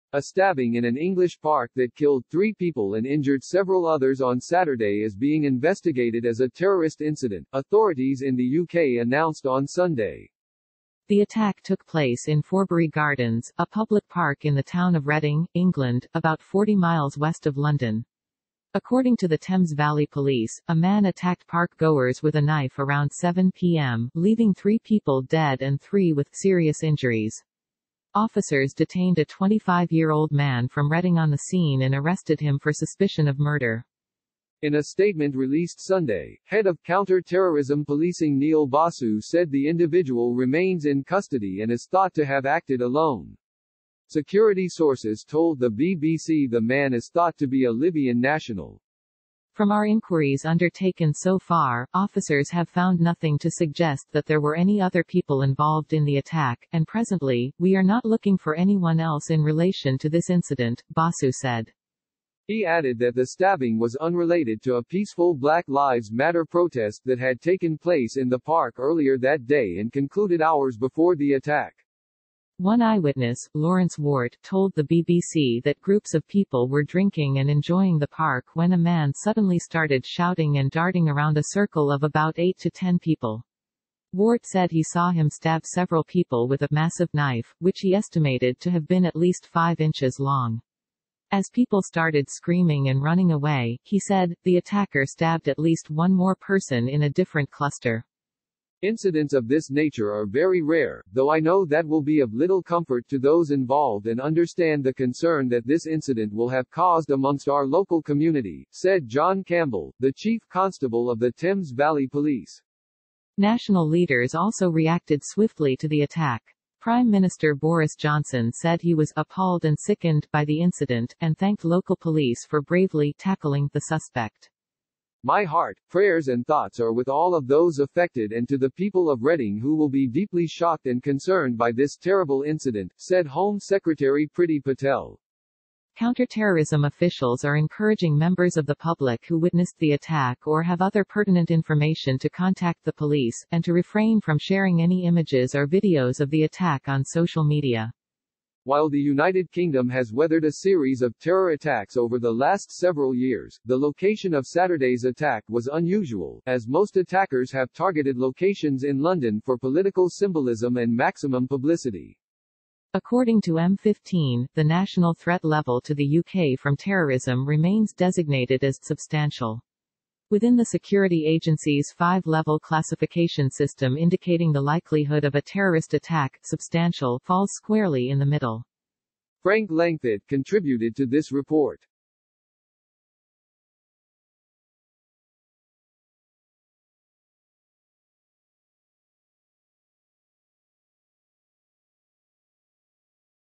Authorities In U.K. Will Investigate English Park Stabbing As A Terrorist Incident
A stabbing in an English park that killed three people and injured several others on Saturday is being investigated as a terrorist incident, authorities in the U.K. announced on Sunday.